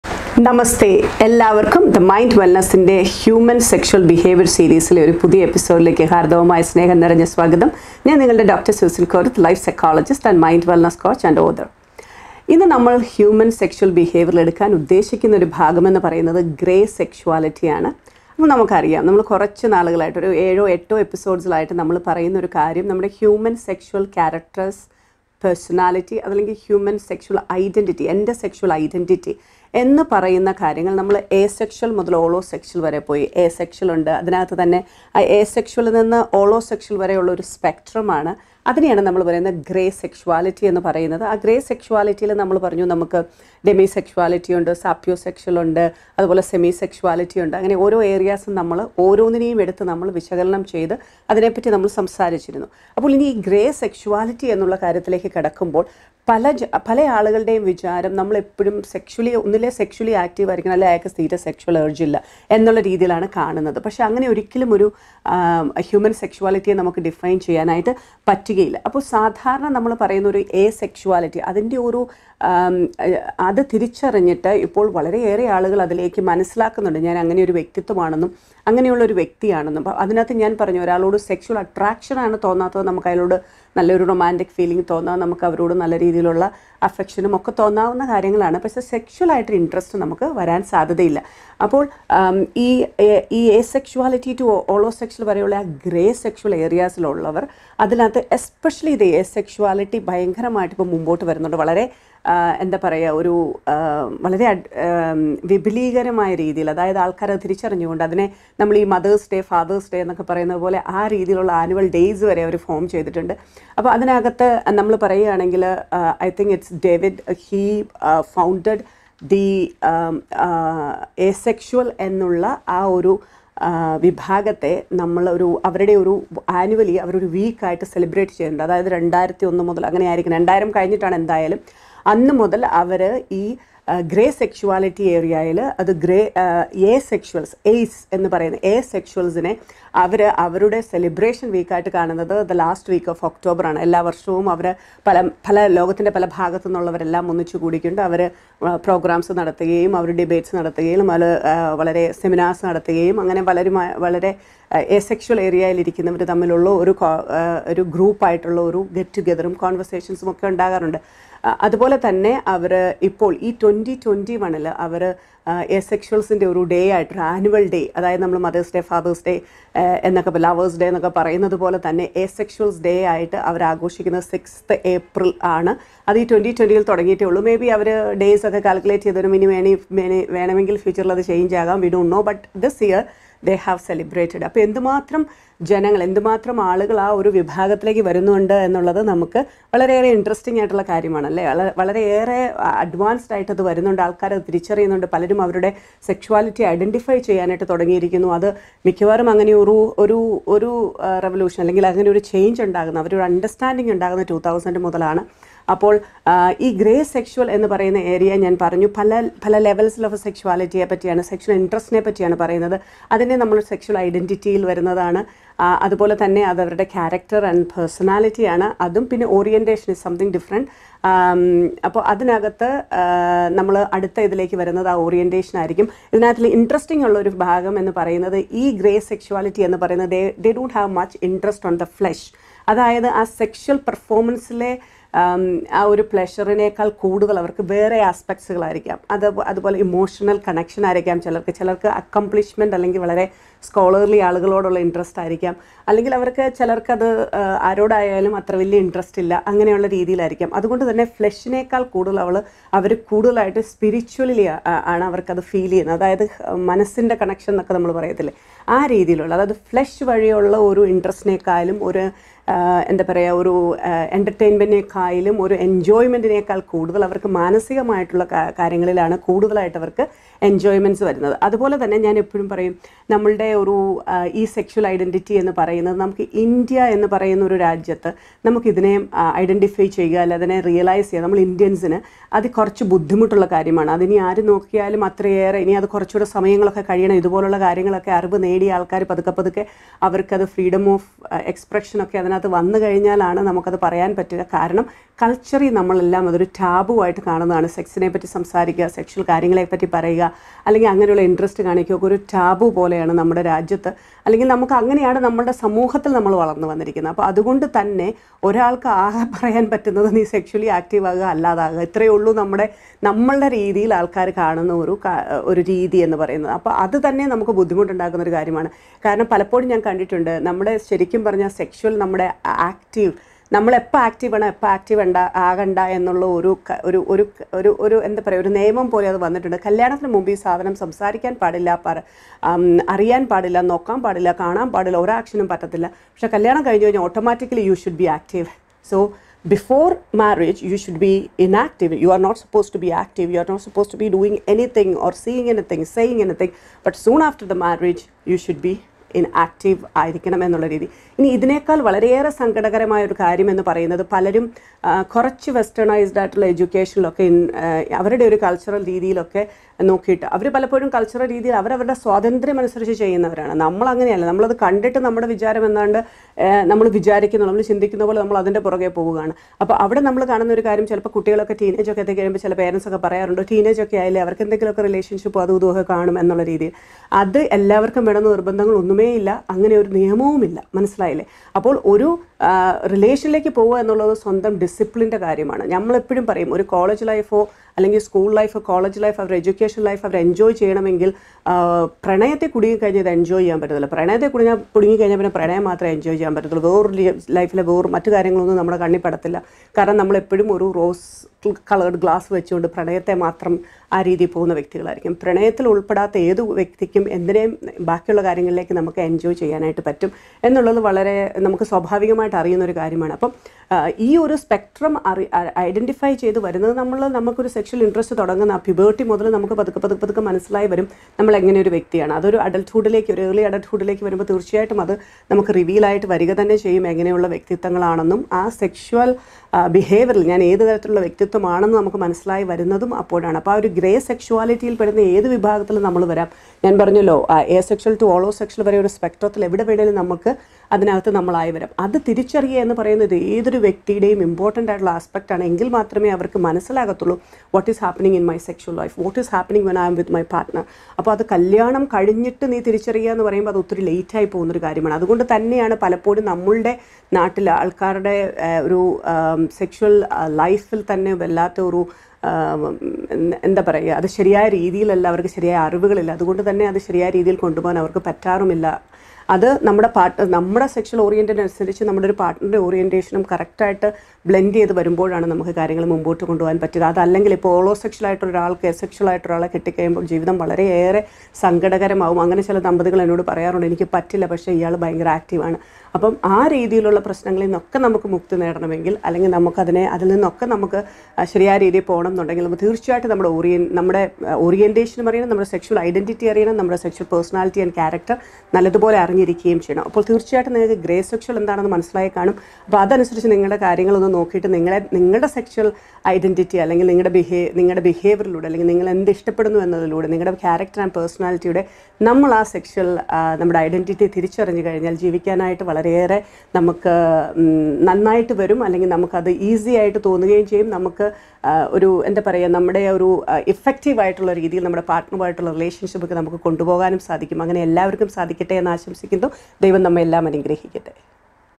HSB Series – Vlog-09 Graysexuality
Welcome to the new episode of mind wellness by Dr Susan Koruthu
‘Graysexuality’ is the seventh episode in the series on human sexual behaviour. The sexuality between asexual and allosexual is called graysexuality. The asexuality and graysexuality communities are getting largely recognised than before as the ACE week celebrated in the last week of October every year since 2010. The first annual asexuality day was celebrated on 6th April this year 2021 to promote asexuality internationally.

The video is made from various research findings and Dr. Susan Koruthu’s experience with a wide range of clients with varied nationalities. The language used is Manglish (a combination of Malayalam and English), as there are not many videos done on this subject in Malayalam.
I hope you will enjoy the video and educate yourself from the information shared in this video.

Make sure you listen to her weekly podcast " The Hidden Power " which is available in:-